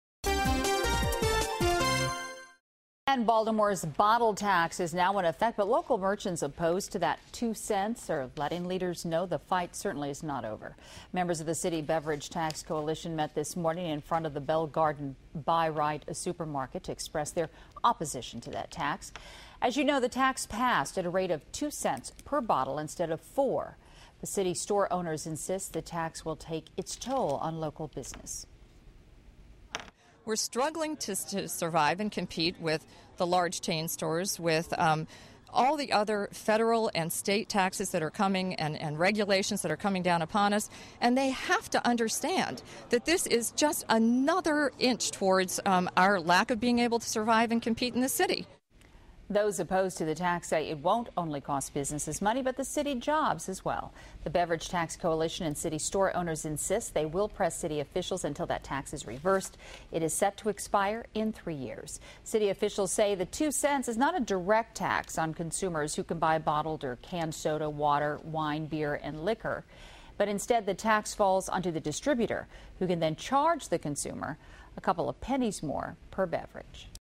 Baltimore 'Bottle Tax' Becomes Official
The much-discussed "bottle tax" is now in place in Baltimore, though opponents haven't stopped voicing their concern.